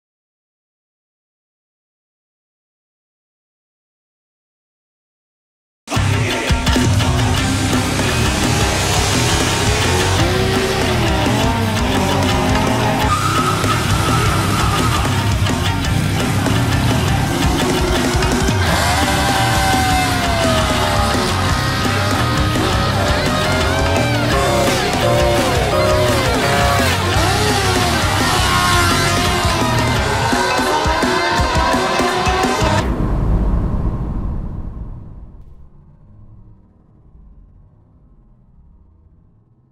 【11月号ふろく映像告知】大加速祭2017
2017年の最新モデル、総勢15台を１０００mの直線路に持ち込み、加速タイムを計測。細かいことは一切抜き！　テスター丸山＆伊丹がとにかく全開＆全開、ひたすら全開！　カテゴリー毎で分けた７つの直線対決を、GPSロガーのデータとともに徹底検証。注目車種の真なるポテンシャルに迫る。